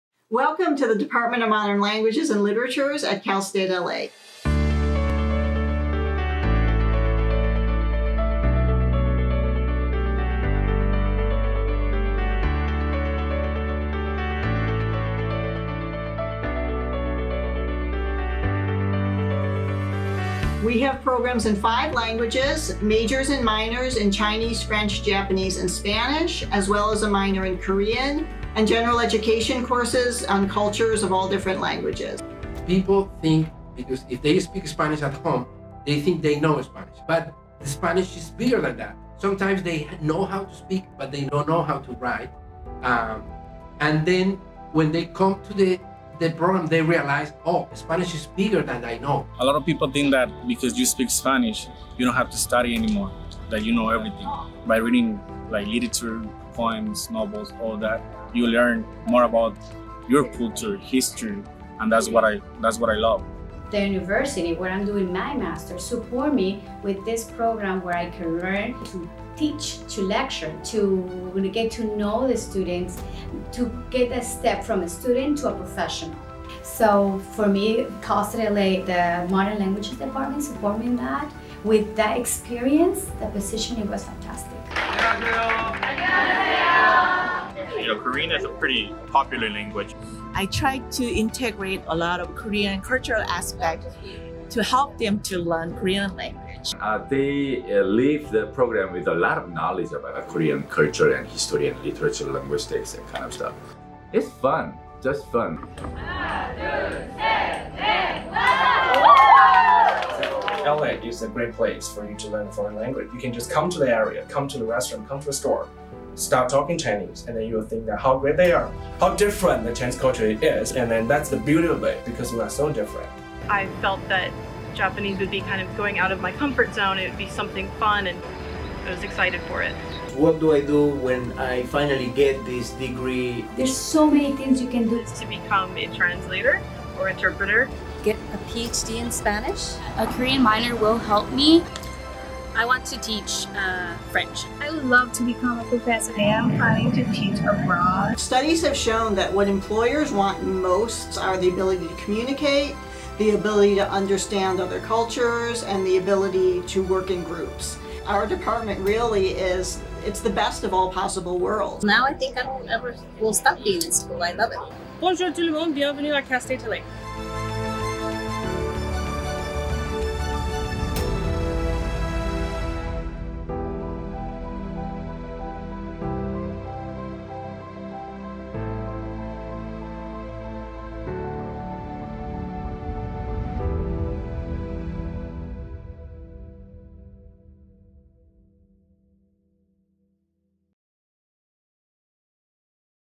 Cal State LA Modern Languages and Literatures Dept.
We invite you to learn more abut our programs from our students & faculty.